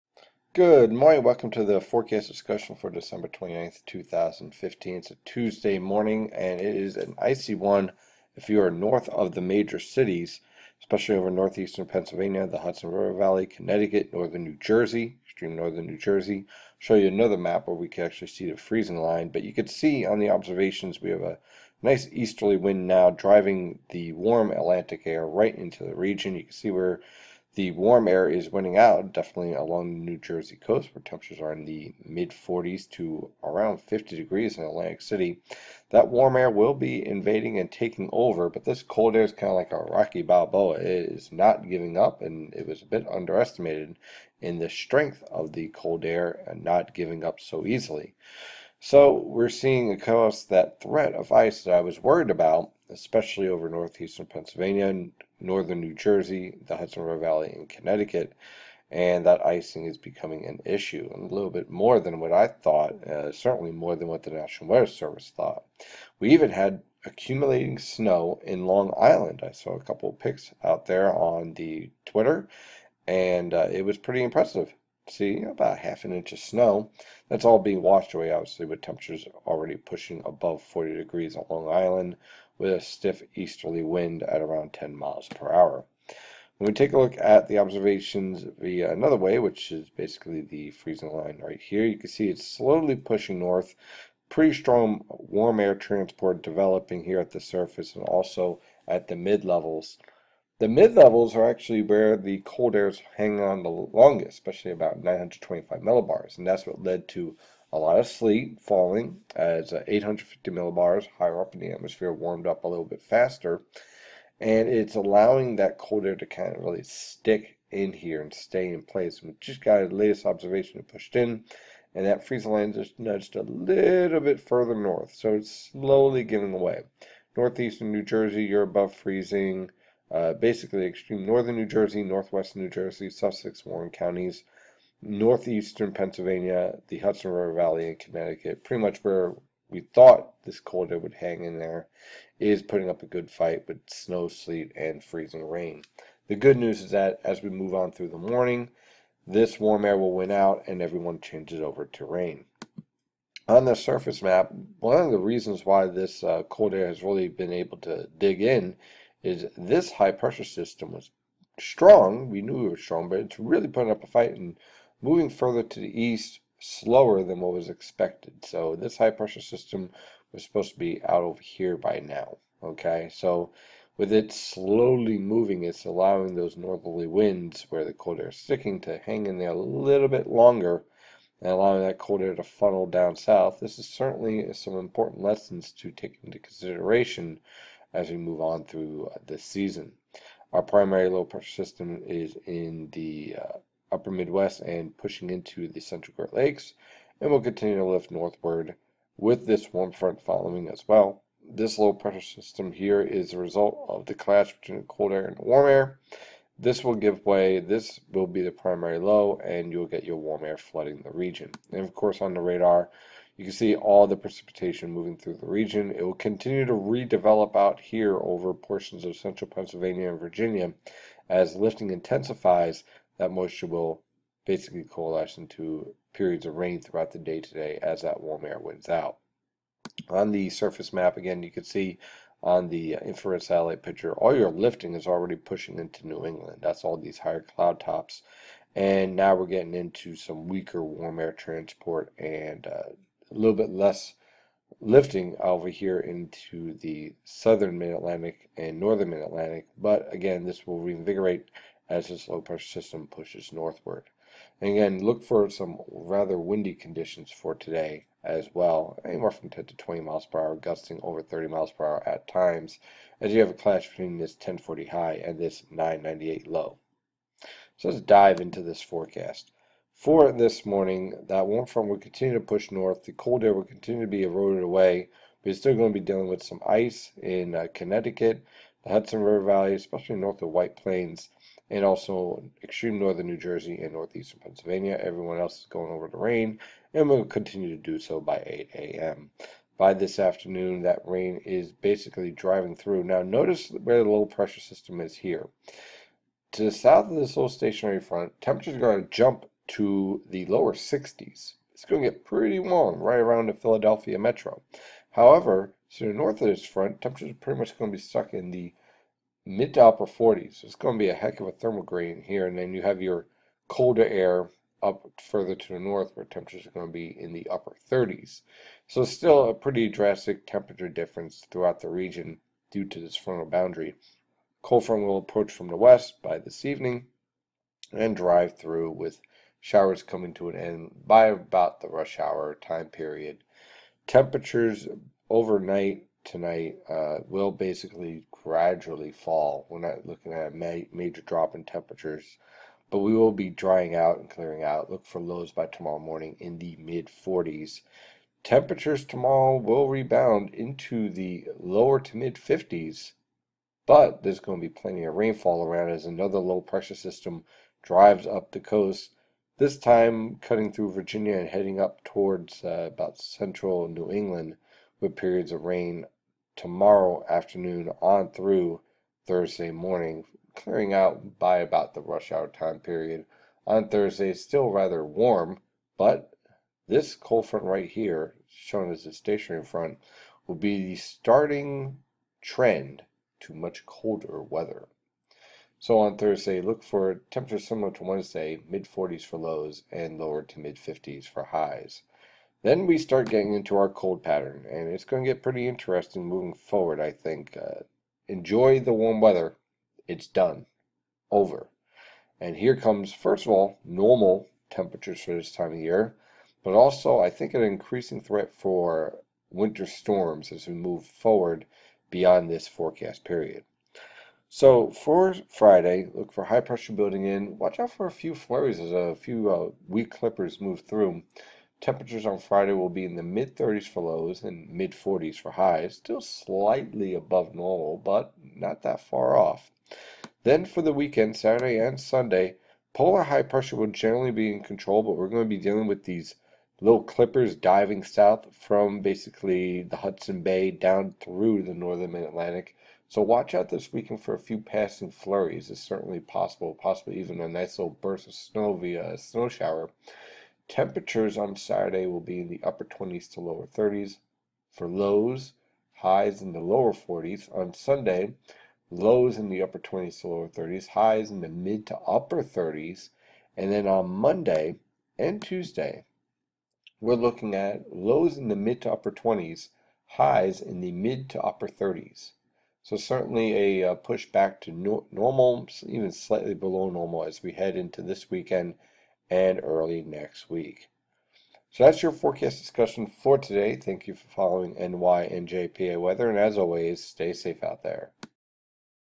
Video Discussion for December 29, 2015
A wintery mix will give way to rain this morning.  Normal weather conditions finally return by this weekend!